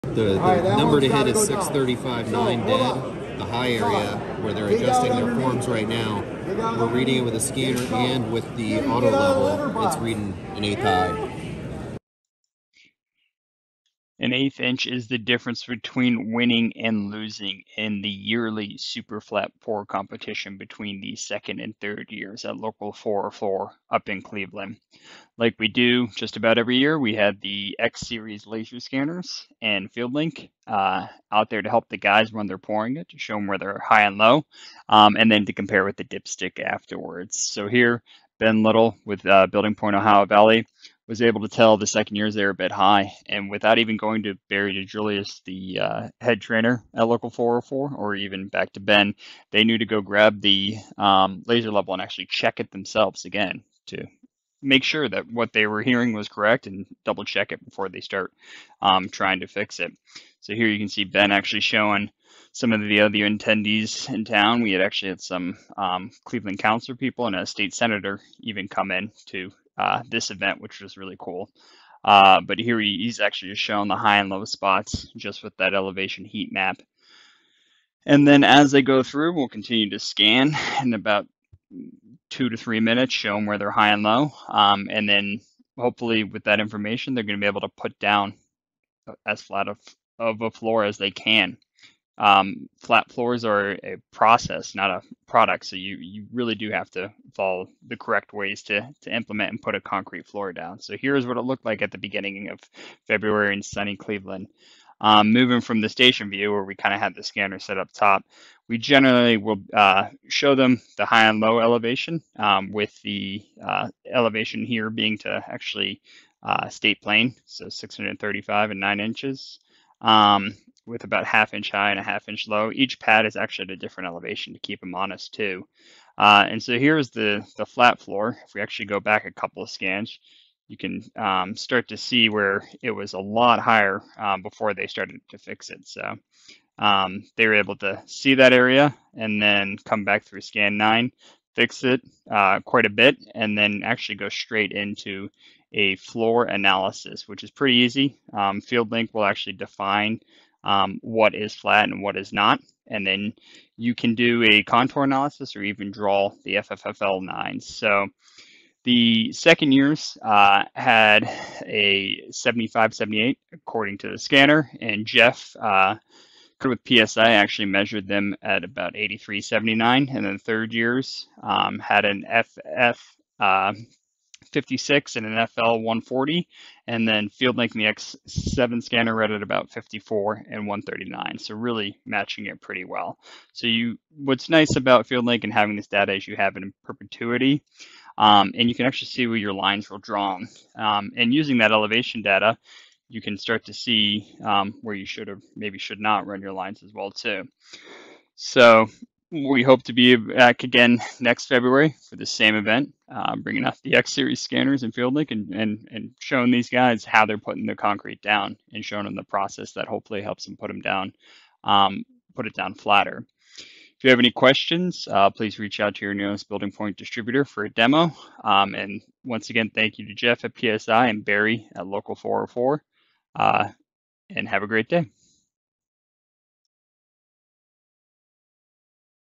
Local 404 Super Flat competition
Local 404 Super Flat competition between 2nd and 3rd year apprentices. Utilizing Trimble FieldLink and X9 Laser Scanner the 2nd-years were able to see a high area with the scan data, compare it with the a level and fix it while the concrete was still wet. Unfortunately an 1/8" was too much to overcome and the 3rd-years won.

This was confirmed with a FF/FL numbers from a D-meter & FieldLink.